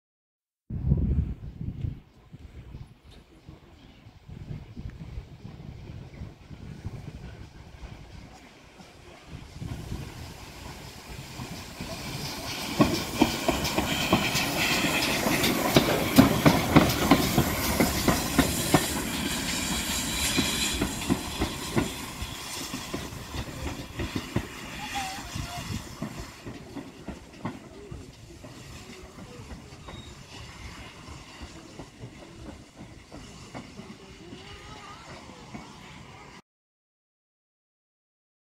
GWR 4555 Small Prairie tank runs round its train at Princes Risborough 24/03/24
alfie_waller_19